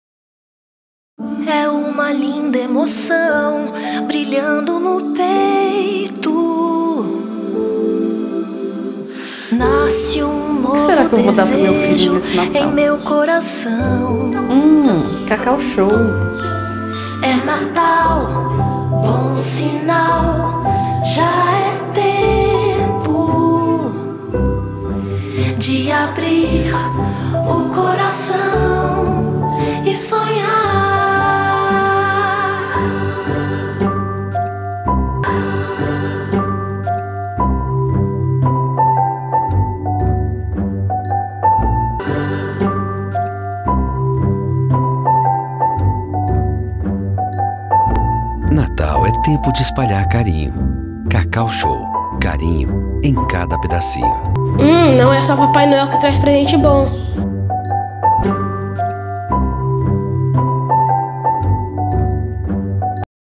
VT Cacau Show Sazonal de Natal - Agência Move
VT Sazonal de Natal, realizado para trabalho acadêmico pela agência Move.